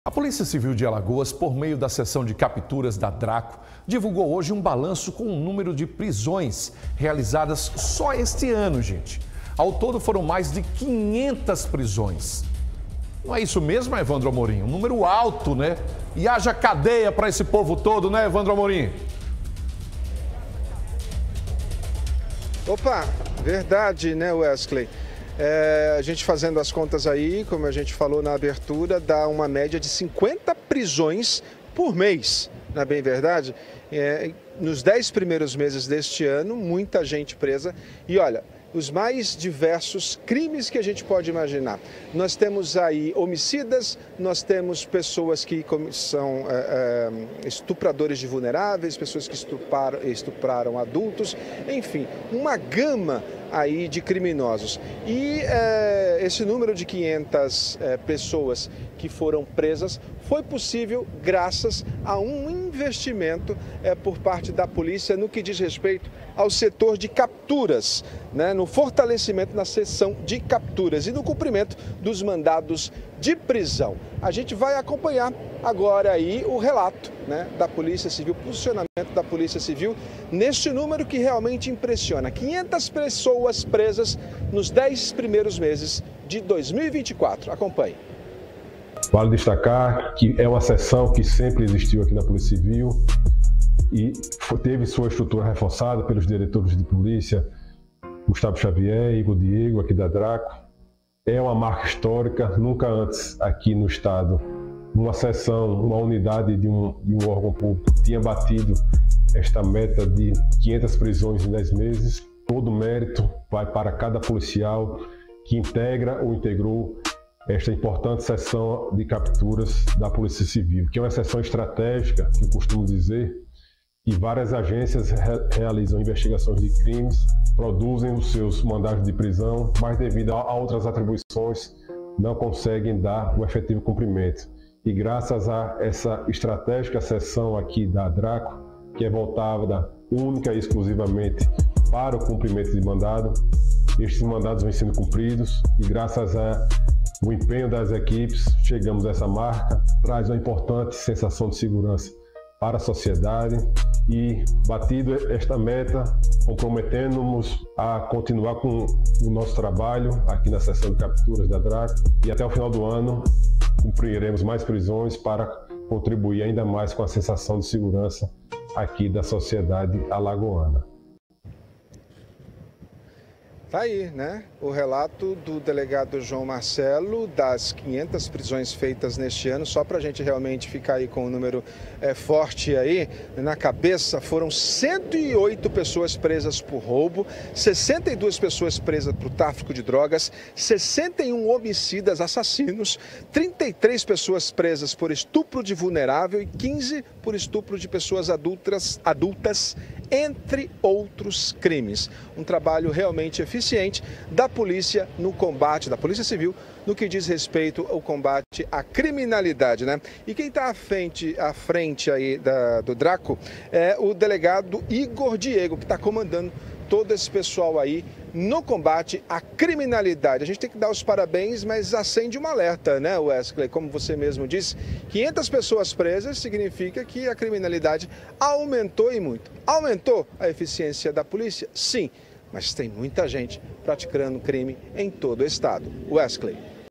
Polícia Civil supera marca de 500 prisões só este ano.
🚨DRACCO | Polícia Civil supera marca de 500 prisões só este ano.